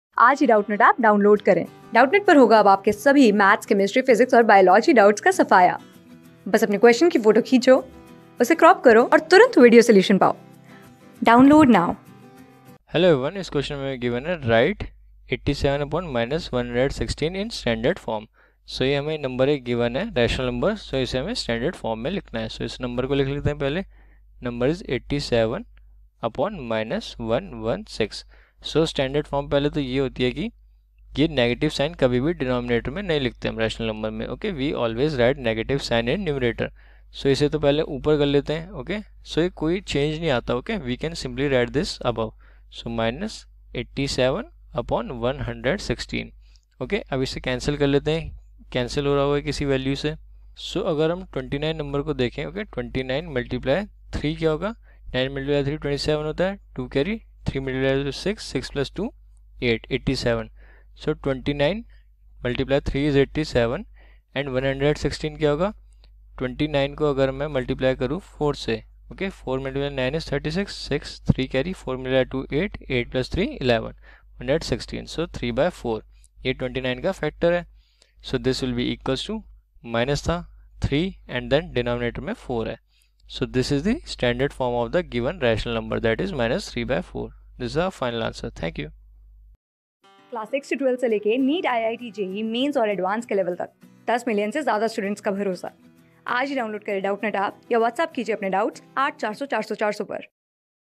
Write (87)/(-116) in standard form. | CLASS 7 | RATIONAL NUMBERS | MATHS | Doubtnut
Write (87)/(-116)  in standard form.
Class: 7
Subject: MATHS
Chapter: RATIONAL NUMBERS
Board:FOUNDATION

👉 Have Any Query? Ask Us.